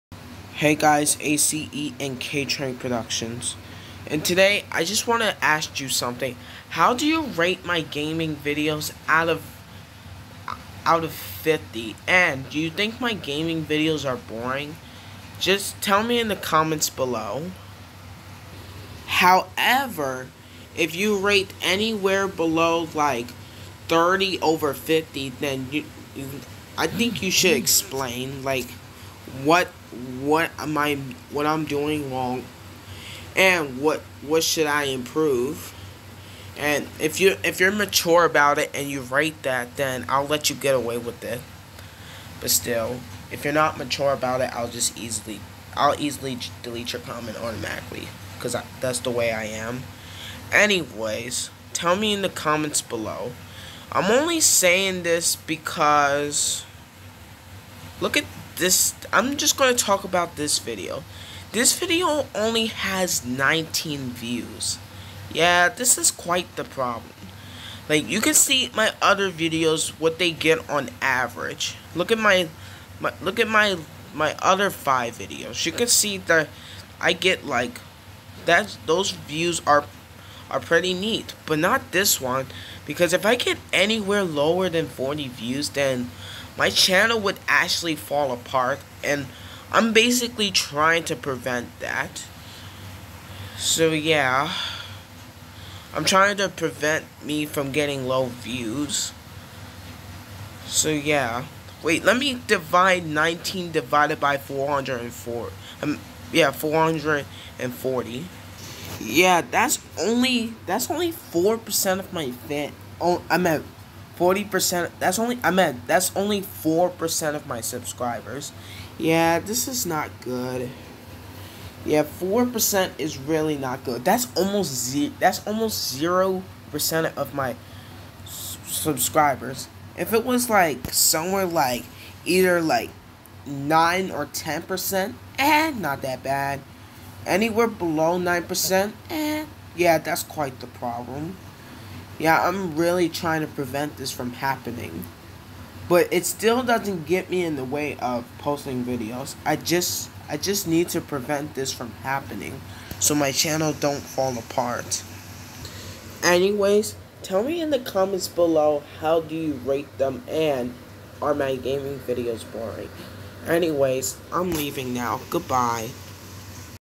Are My Gaming Videos Boring? How Do You Rate Them Out Of 50? Watch The Whole Video
I'm only saying this because less and less people are watching my gaming Videos I Post 2 Mario Kart 8 Videos They Could Barely Reach 20 Views But Deleted Those Videos